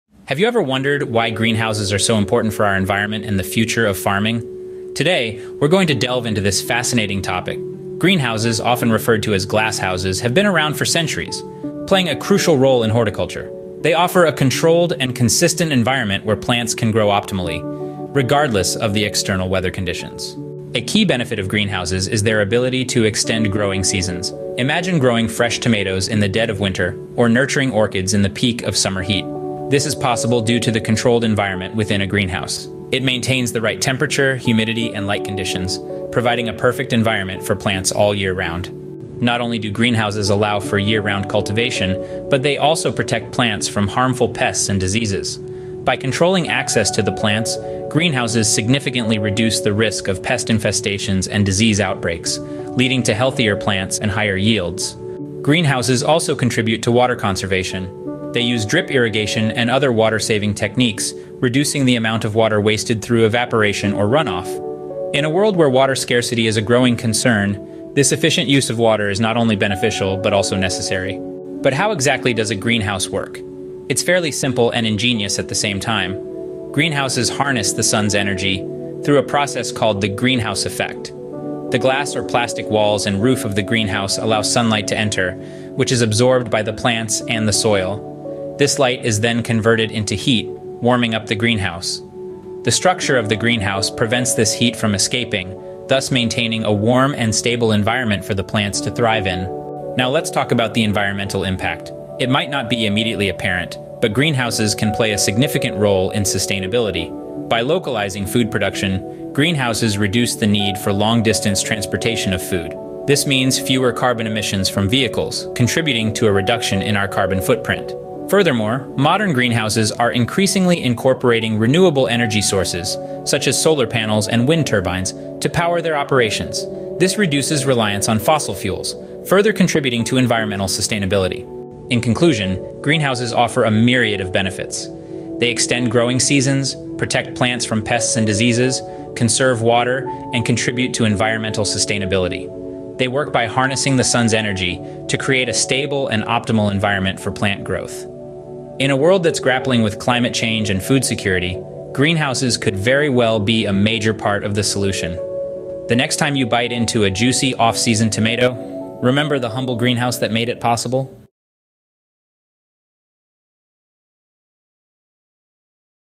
The Green House Effect: A Detailed Overview
Join us in this informative video as we dive into the fascinating topic of the greenhouse effect. In this video, we will explain what the greenhouse effect is, its causes, and most importantly, its impact on our planet. 🌍

Discover the science behind this phenomenon as we explore how certain gases trap heat in the Earth's atmosphere, leading to climate change and rising temperatures. Our easy-to-understand explanations will help you grasp the importance of understanding and addressing this global issue.

Learn how human activities such as burning fossil fuels, deforestation, and industrialization contribute to the intensification of the greenhouse effect. Gain insights into the consequences of unchecked greenhouse gas emissions and how they can disrupt ecosystems, weather patterns, and cause sea-level rise.

By the end of this video, you'll have a clear understanding of why taking action to reduce greenhouse gas emissions is crucial for the future of our planet. Together, we can make a difference! 🔥🌿

OUTLINE:

00:00:00 Introduction to Greenhouses
00:00:20 Extending Growing Seasons
00:00:38 Protection from Pests and Diseases
00:00:55 Water Conservation
00:01:11 How Greenhouses Work
00:01:39 Environmental Impact
00:02:11 Conclusion